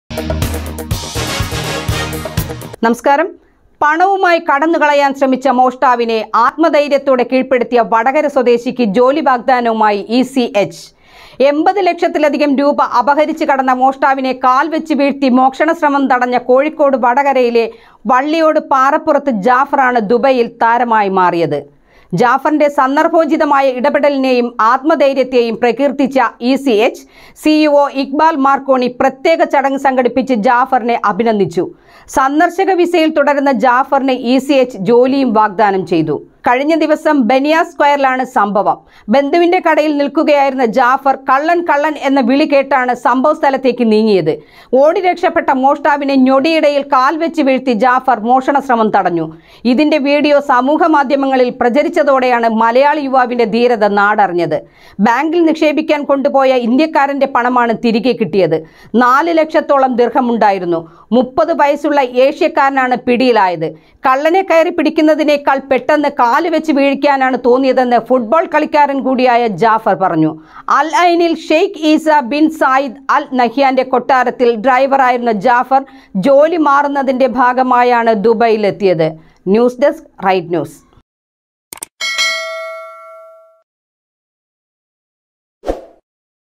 ഈസാ ബിൻ സായിദ് അൽ നഹ്യാന്റെ കൊട്ടാരത്തിൽ ഡ്രൈവറായിരുന്നു ജാഫർ
പണവുമായി കടന്നുകളയാന്‍ ശ്രമിച്ച മോഷ്ടാവിനെ ആത്മധൈര്യത്തോടെ കീഴ്‌പ്പെടുത്തിയ വടകര സ്വദേശിക്ക് ജോലി വാഗ്ദാനവുമായി ഇ.സി.എച്ച്. 80 ലക്ഷത്തിലധികം രൂപ അപഹരിച്ച് കടന്ന മോഷ്ടാവിനെ കാല്‍വെച്ച് വീഴ്ത്തി മോഷണശ്രമം തടഞ്ഞ കോഴിക്കോട് വടകരയിലെ വള്ളിയോട് പാറപ്പുറത്ത് ജാഫറാണ് ദുബൈയില്‍ താരമായി മാറിയത്. ജാഫറിന്റെ സന്ദര്‍ഭോചിതമായ ഇടപെടലിനെയും ആത്മധൈര്യത്തെയും പ്രകീര്‍ത്തിച്ച ഇ.സി.എച്ച് സി.ഇ.ഒ ഇഖ്ബാല്‍ മാര്‍ക്കോണി പ്രത്യേക ചടങ്ങ് സംഘടിപ്പിച്ച് ജാഫറിനെ അഭിനന്ദിച്ചു. സന്ദര്‍ശക വിസയില്‍ തുടരുന്ന ജാഫറിന് ഇ.സി.എച്ച് ജോലിയും വാഗ്ദാനം ചെയ്തു.

കഴിഞ്ഞദിവസം ബനിയാസ് സ്‌ക്വയറിലാണ് സംഭവം. ബന്ധുവിന്റെ കടയില്‍ നില്‍ക്കുകയായിരുന്ന ജാഫര്‍ കള്ളന്‍, കള്ളന്‍ എന്ന വിളി കേട്ടാണ് സംഭവസ്ഥലത്തേക്ക് നീങ്ങിയത്. ഓടിരക്ഷപ്പെട്ട മോഷ്ടാവിനെ ഞൊടിയിടയില്‍ കാല്‍വെച്ചു വീഴ്ത്തി ജാഫര്‍ മോഷണശ്രമം തടഞ്ഞു. ഇതിന്റെ വിഡിയോ സമൂഹ മാധ്യമങ്ങളില്‍ പ്രചരിച്ചതോടെയാണ് മലയാളി യുവാവിന്റെ ധീരത നാടറിഞ്ഞത്. ബാങ്കില്‍ നിക്ഷേപിക്കാന്‍ കൊണ്ടുപോയ ഇന്ത്യക്കാരന്റെ പണമാണ് തിരികെ കിട്ടിയത്. നാലു ലക്ഷത്തോളം ദിര്‍ഹമുണ്ടായിരുന്നു. 30 വയസ്സുള്ള ഏഷ്യക്കാരനാണ് പിടിയിലായത്. കള്ളനെ കയറിപ്പിടിക്കുന്നതിനേക്കാള്‍ പെട്ടെന്ന് കാലുവെച്ച് വീഴിക്കാനാണ് തോന്നിയതെന്ന് ഫുട്ബാള്‍ കളിക്കാരന്‍കൂടിയായ ജാഫര്‍ പറഞ്ഞു. അല്‍ ഐനില്‍ ഷെയ്ഖ് ഈസാ ബിന്‍ സായിദ് അല്‍ നഹ്യാന്റെ കൊട്ടാരത്തില്‍ ഡ്രൈവറായിരുന്ന ജാഫര്‍ ജോലി മാറുന്നതിന്റെ ഭാഗമായാണ് ദുബൈയിലെത്തിയത്